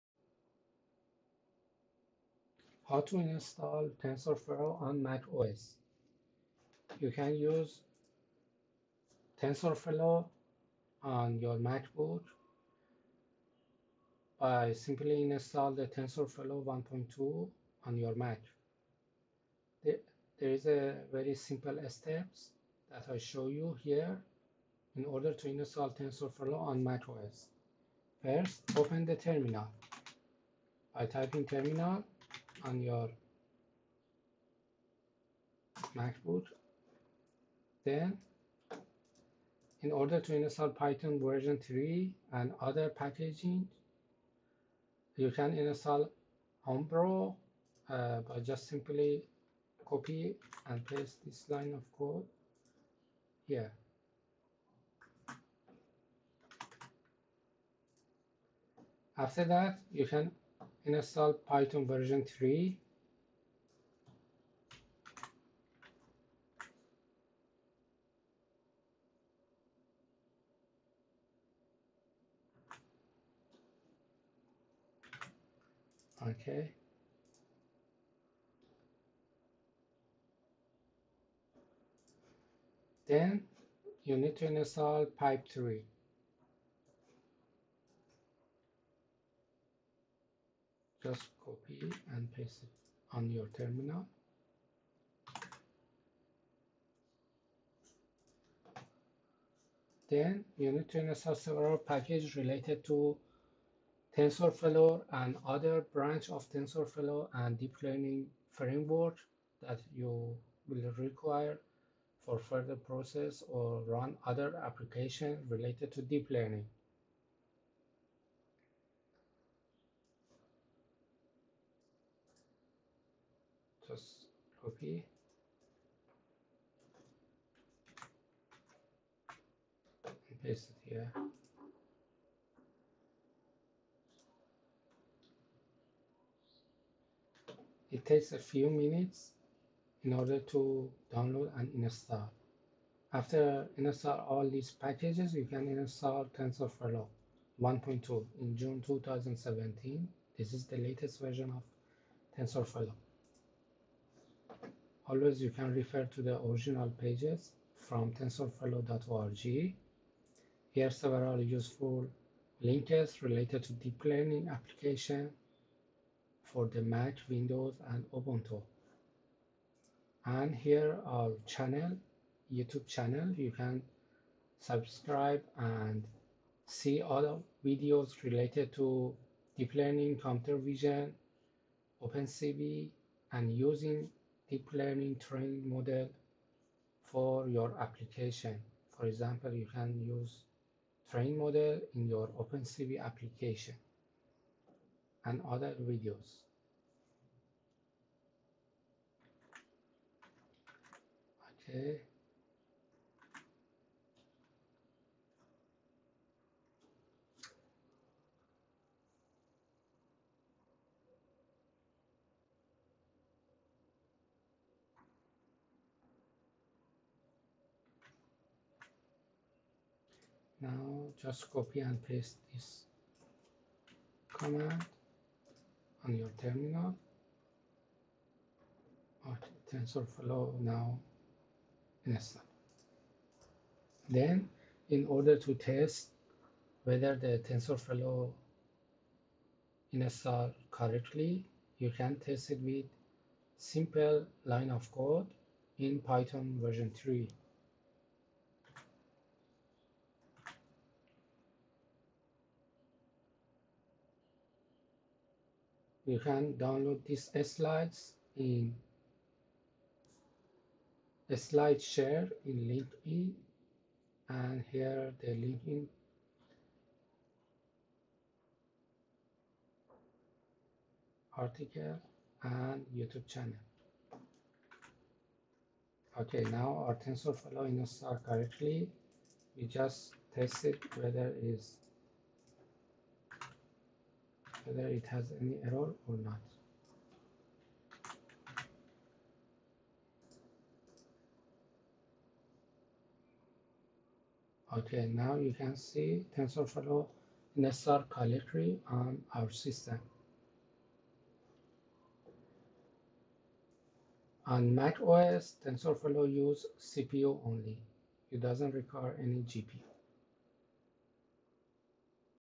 Deep Learning on MacOS (MacBook) with TensorFlow (June 2017): Part 1
Deep Learning on MacOS (MacBook) with TensorFlow (June 2017)

Part 1Install TensorFlow 1.2Youtube.com/tiziran
Deep Learning on MacOS
Install Homebrew

Install python 3
brew install python3

Install pip3
sudo easy_install --upgrade pip3

sudo easy_install --upgrade  six

Install TensorFlow 1.2 (June 2017)
sudo -H pip3 install --upgrade --force-reinstall tensorflow

Test
python3

import tensorflow as tf
hello = tf.constant('Hello, TensorFlow!')
sess = tf.Session()
print(sess.run(hello))

Reference
Install TensorFlow 1.2 on macOS Sierra on MacBook (June 2017)